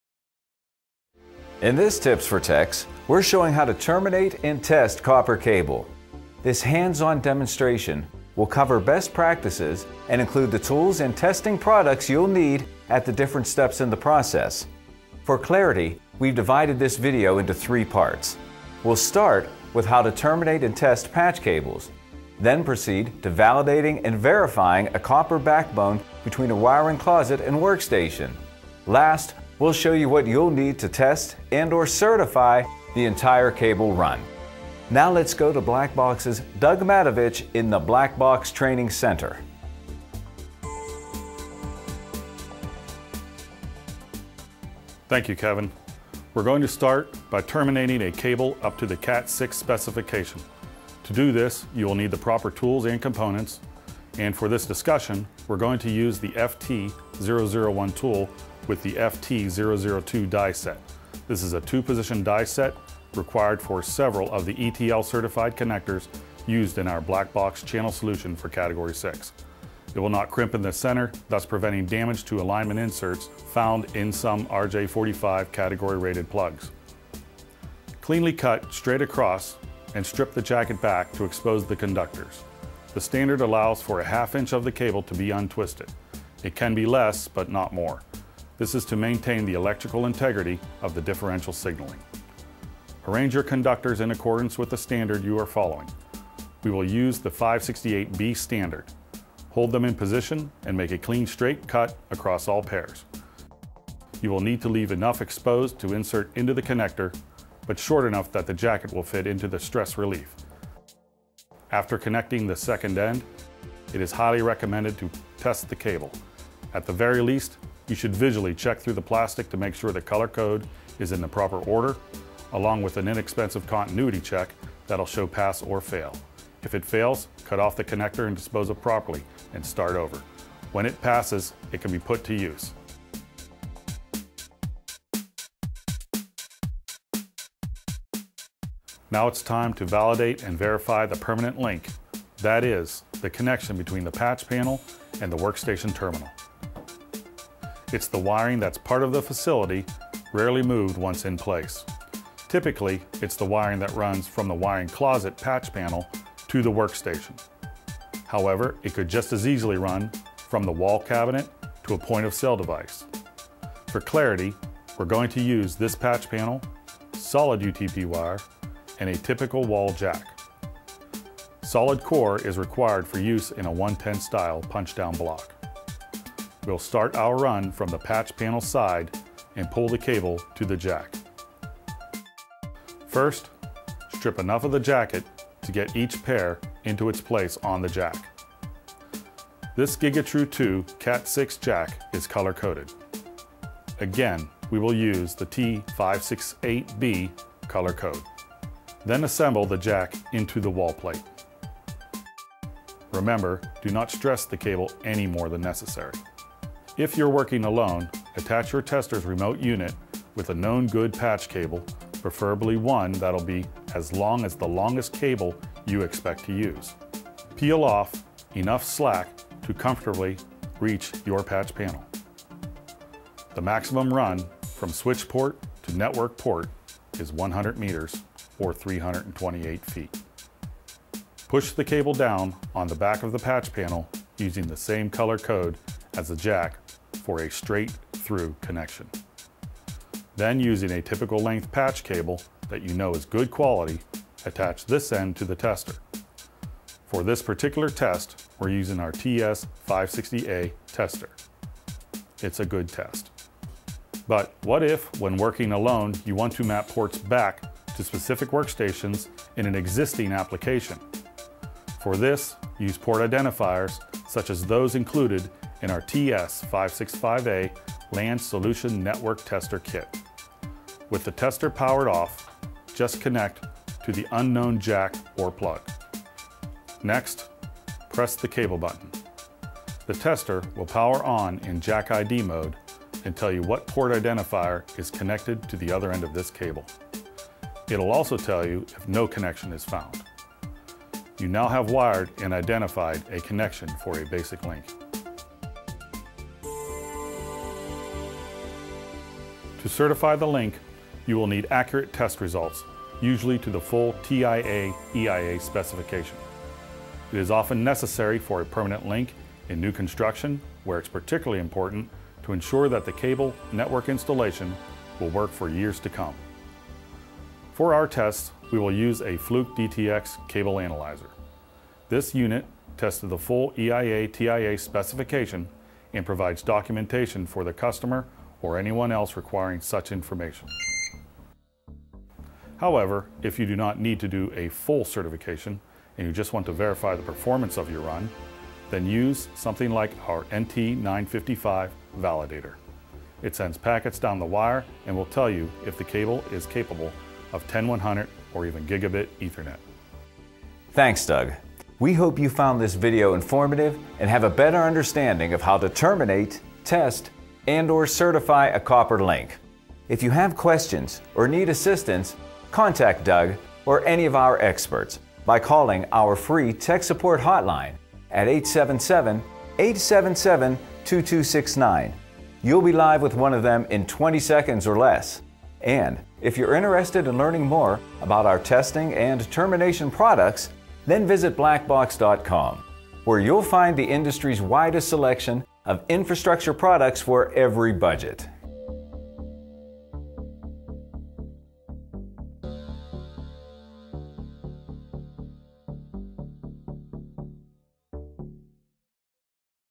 How to Terminate and Test Copper Cable | Black Box®
| This hands-on Tips for Techs video takes you inside the Black Box Training Center to show you how to terminate and test cabling for a copper installation. The video begins with a step-by-step demonstration of terminating a patch cable up to the CAT6 specification, with special focus on how to cut and strip the cabling in a way that ensures good signaling integrity. Next, it covers how to validate and verify a backbone link between a patch panel and workstation. The video tutorial then demonstrates what kind of tester you'll need if you must certify and document a copper link, then ends with a discussion on what you'll need to simply verify the Ethernet performance of a cable run.